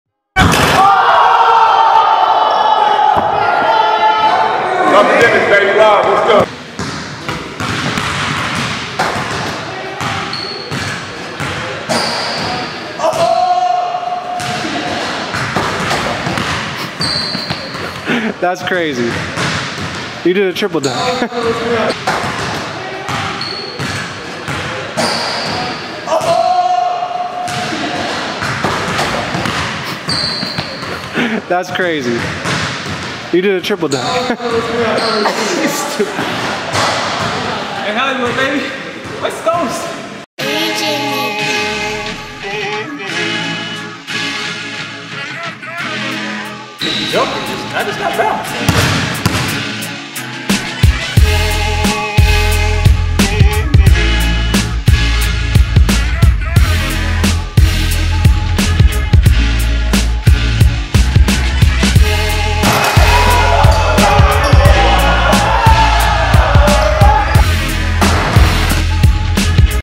The ULTIMATE H.O.R.S.E. Shot! The Triple Dunk!
Young Hollywood goes Off the Wall for a two hand dunk, catches the ball and dunks with his FEET, then catches it again for a one hand Slam!